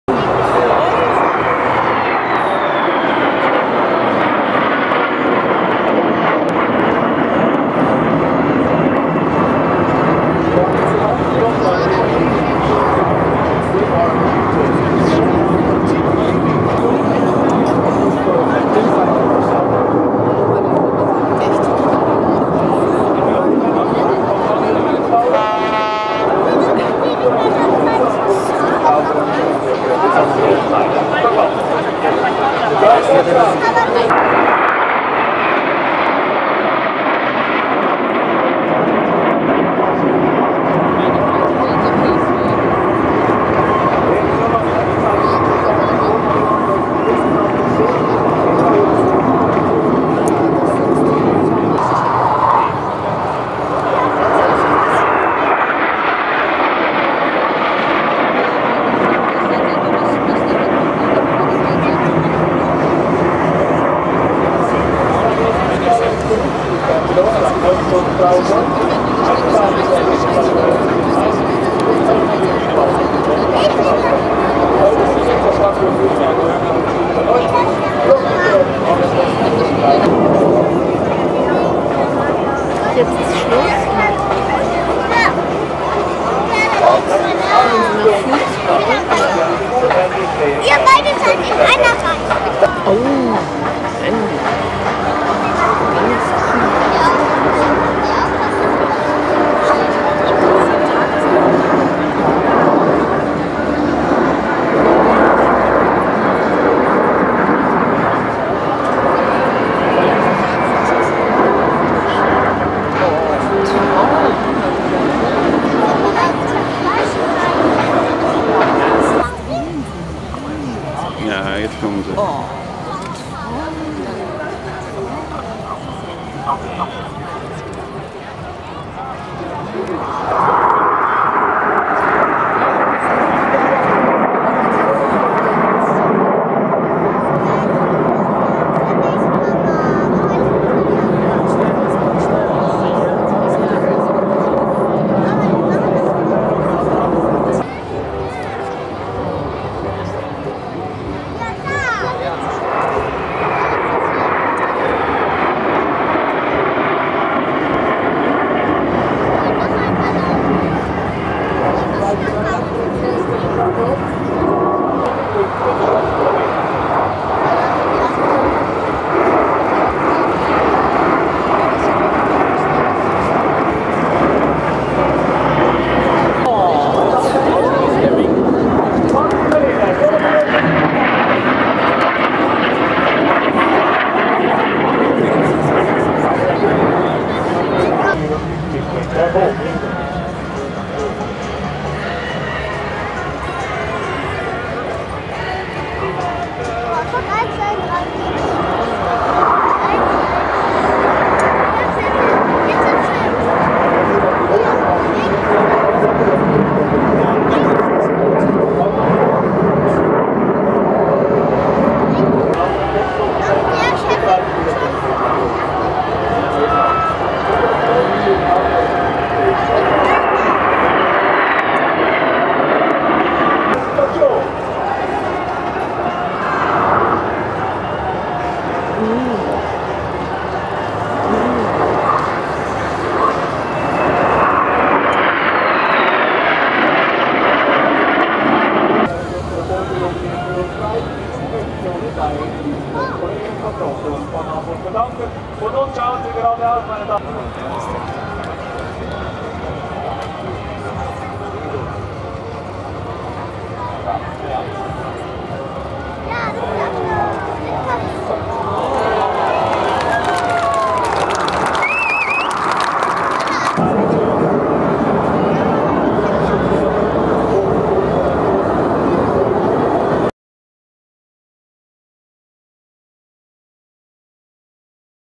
Patrouille Suisse
Schweizer Kunstflugstaffel über Hamburger Hafen / Aerobatic Team of Swiss Air Force at Hamburg